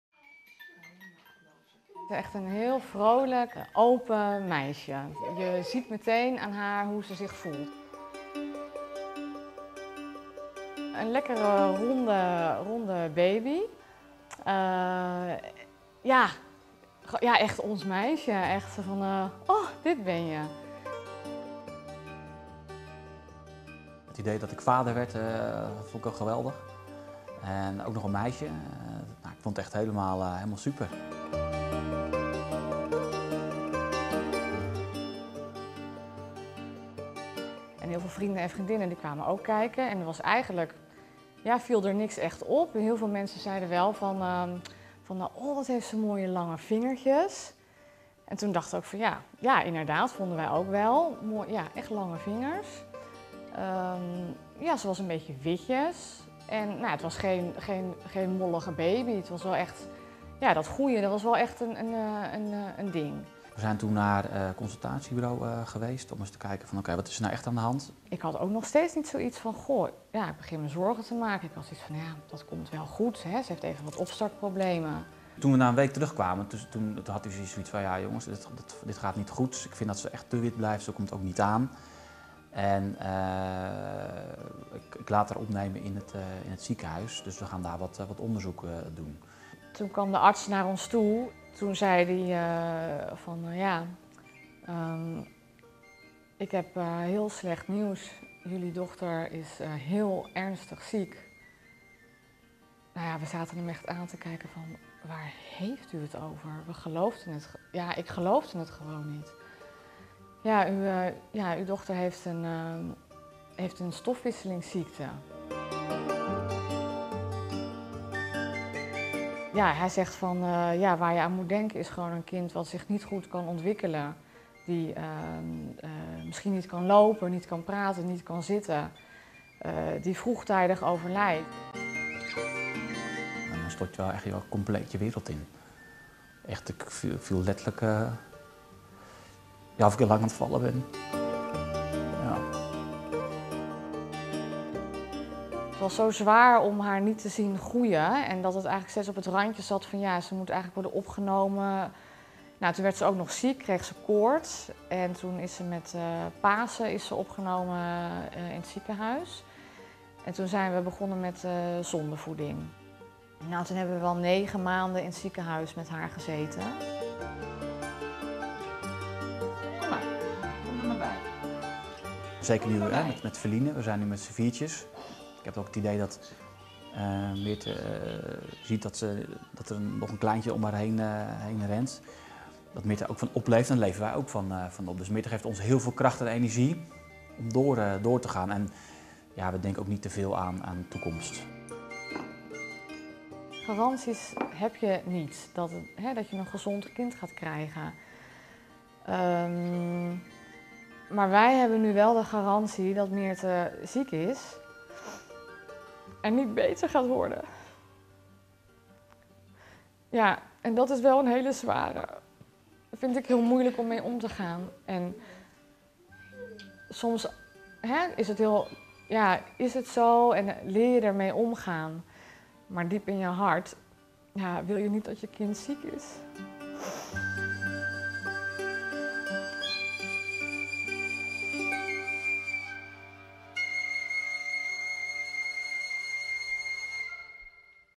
Metakids portret: Myrthe
Marit en Wybo Jan vertellen in dit videoportret over de grote impact die de metabole ziekte van Myrthe op hun gezin heeft. Lees ook hun verhaal